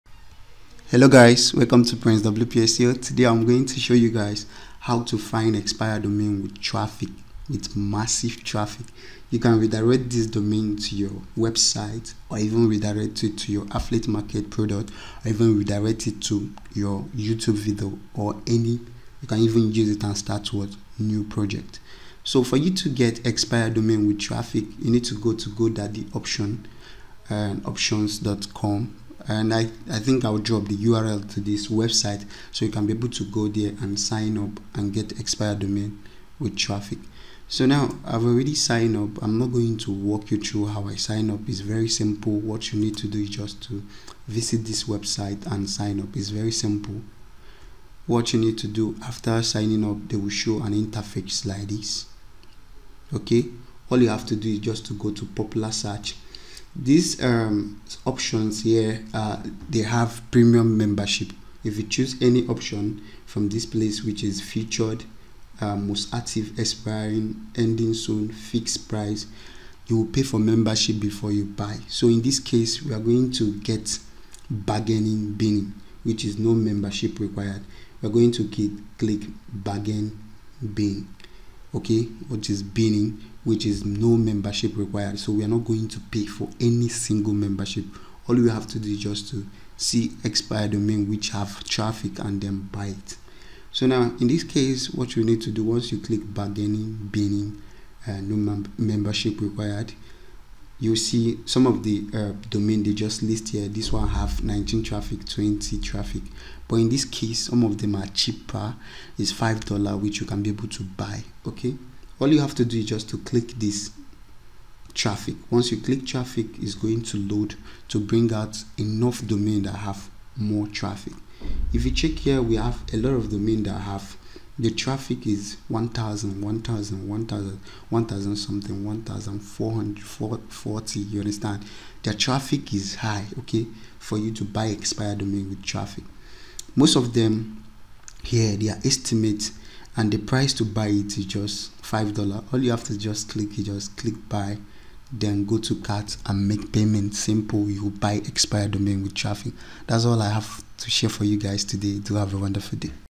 How To Find Expired Domains With Traffic 2021
do you want to know How To Find Expired Domains With Traffic 2021, then this video will guide you on how to find a powerful expired domain with traffic.

we all know the importance of expired domain, an expired domain can be use it for SEO purpose which will you boost your ranking and traffic.